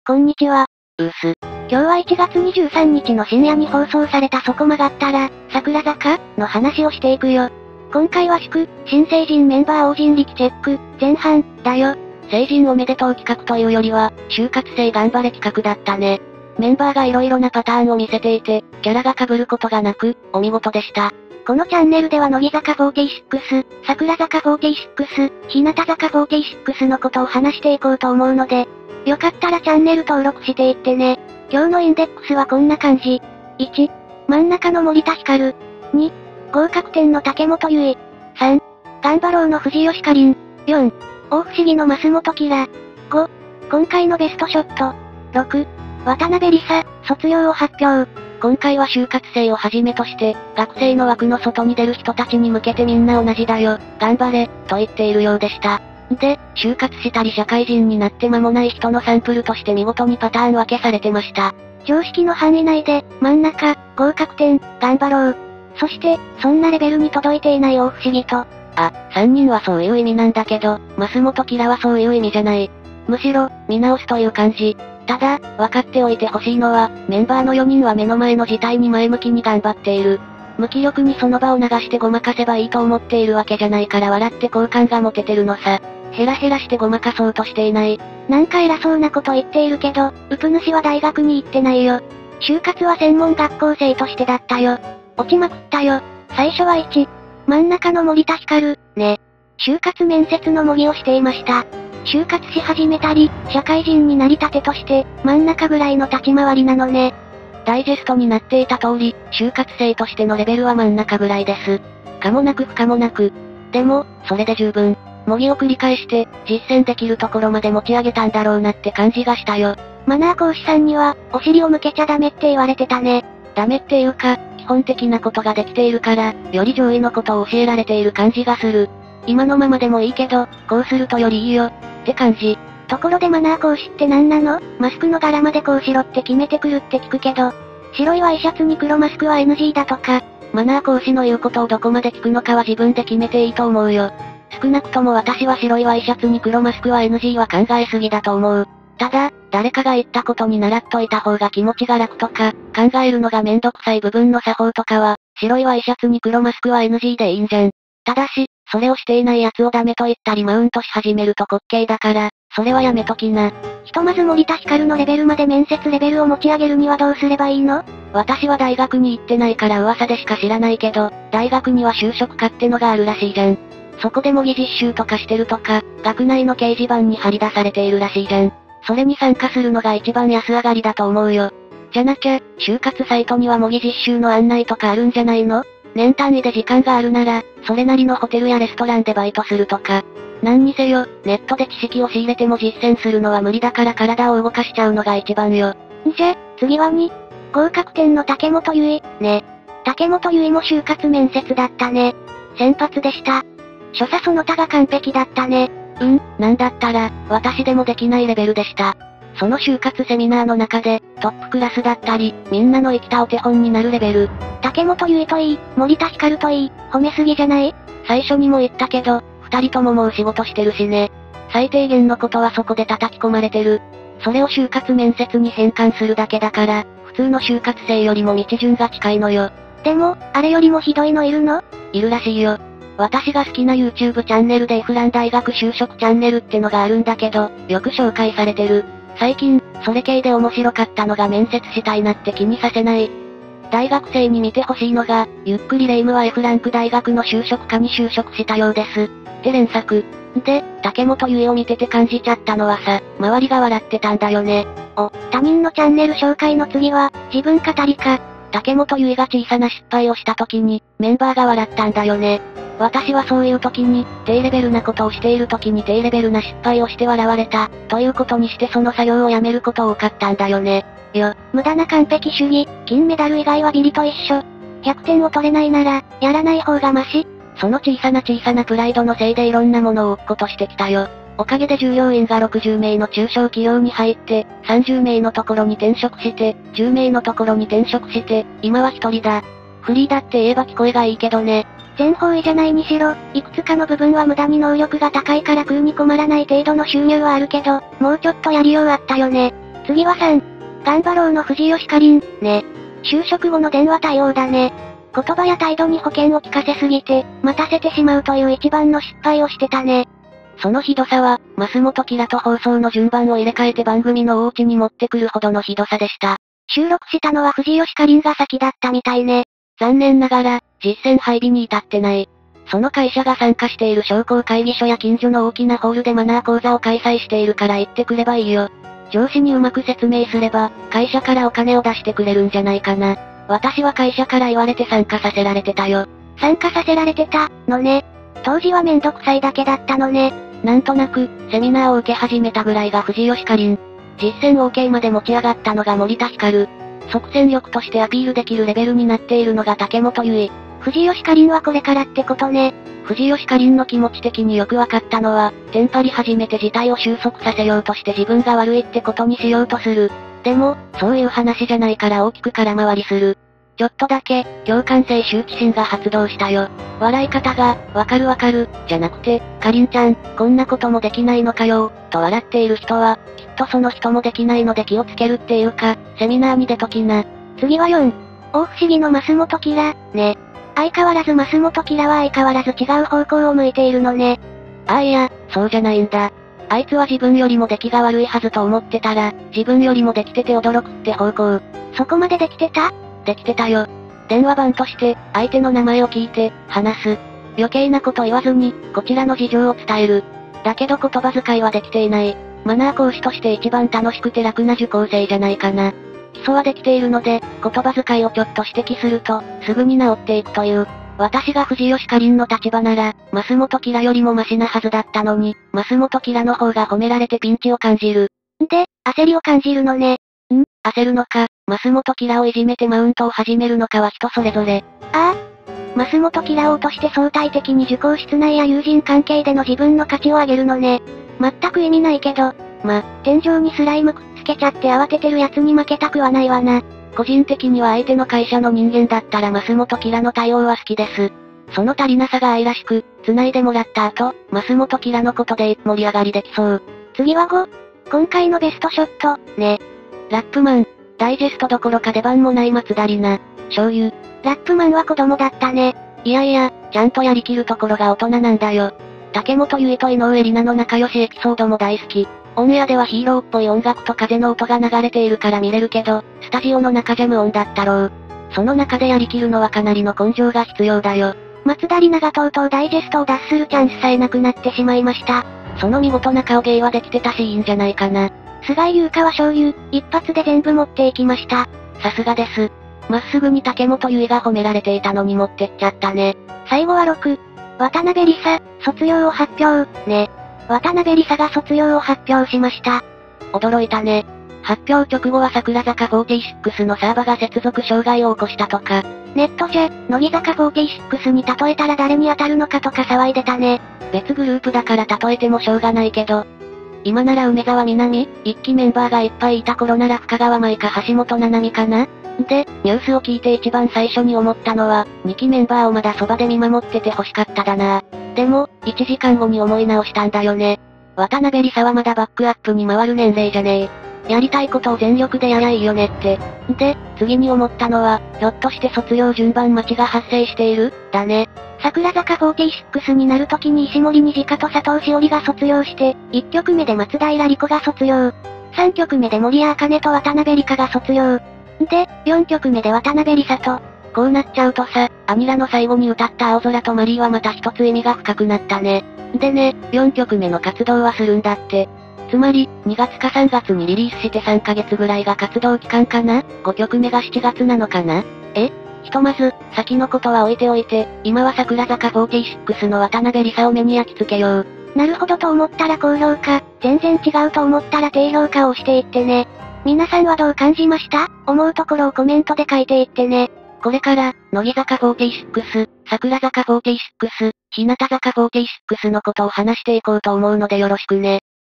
【そこさく】01/23「祝！新成人メンバー大人力チェック！前半」のお話【櫻坂46】
01/23のそこ曲がったら、櫻坂の
「祝！新成人メンバー大人力チェック！前半」のレビューです。

■インデックス
00:00 前説
01:36 1.真ん中の森田ひかる
03:16 2.合格点の武元唯衣
05:00 3.頑張ろうの藤吉夏鈴
06:18 4.大不思議の増本綺良
07:27 5.今回のベストショット
08:07 6.渡邉理佐、卒業を発表